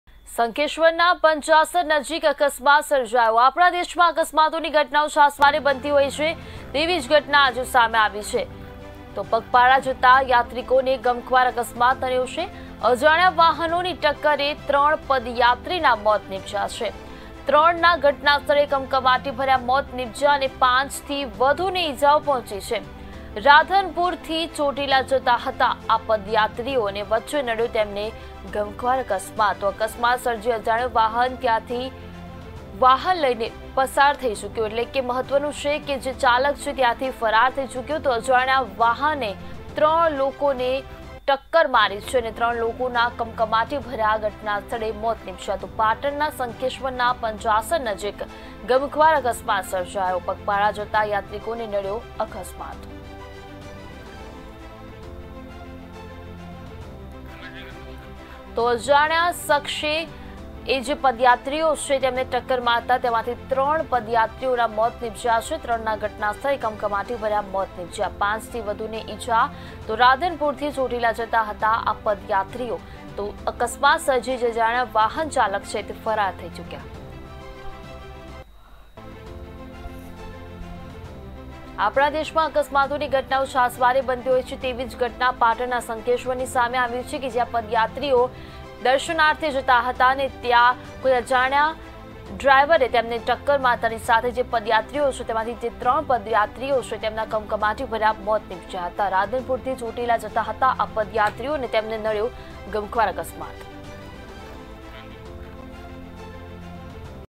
PATAN : અકસ્માતનો સિલસિલો યથાવત
પાટણઃ શંખેશ્વરના પંચાસર નજીક અકસ્માત
પગપાળા જતા યાત્રિકોને નડ્યો અકસ્માત
અજાણ્યા વાહનની ટક્કરે 3 પદયાત્રીના મોત
3નાં ઘટનાસ્થળે મોત અને 5થી વધુને ઈજા
રાધનપુરથી ચોટીલા જતા હતા પદયાત્રીઓ
અકસ્માત સર્જી અજાણ્યો વાહન ચાલક ફરાર